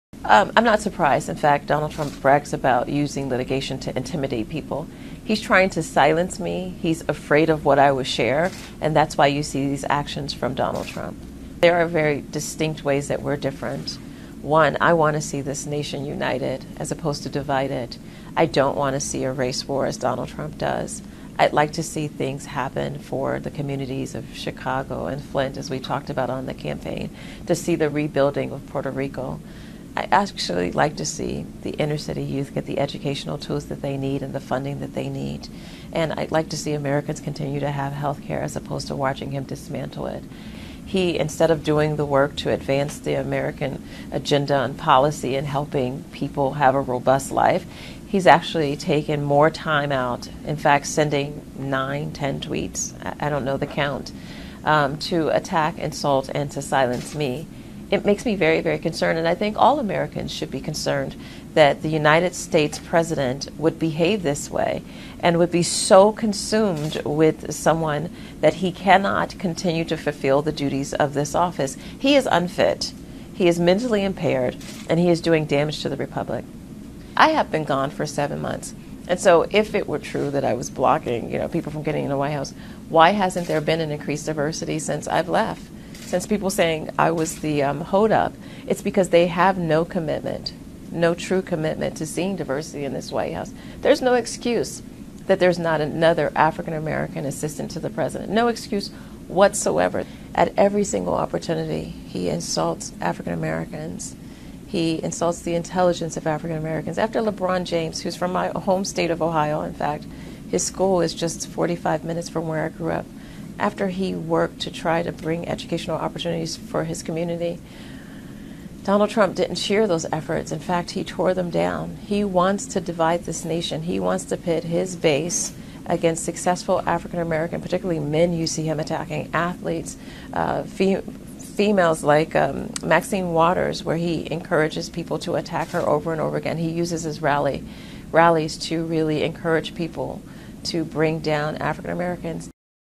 Former White House Aide Says Trump Wants A 'Race War'
August 14, 2018 - Former White House aide Omarosa Manigault Newman is declaring she "will not be silenced" by President Donald Trump, remaining defiant as her public feud with her former boss shifts from a war of words to a possible legal battle. (Aug. 14)